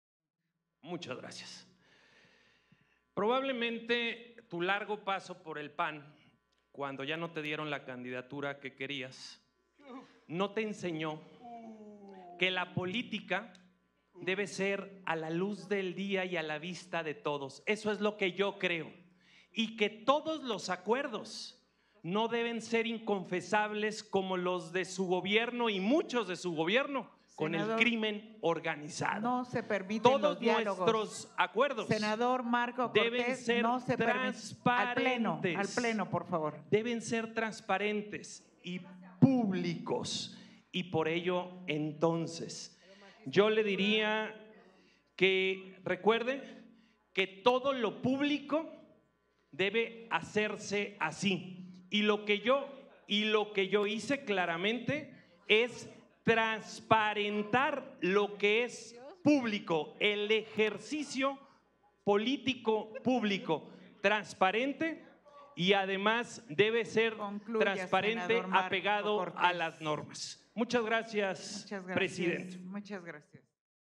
Senador Marko Cortés Mendoza, por hechos, en la discusión de la minuta que modifica la Constitución
8 de septiembre de 2024.- Intervención del senador Marko Cortés Mendoza, por hechos, en la discusión de la minuta que modifica la Constitución en materia de reforma del Poder Judicial, durante la reunión de las comisiones unidas de Puntos Constitucionales y de Estudios Legislativos.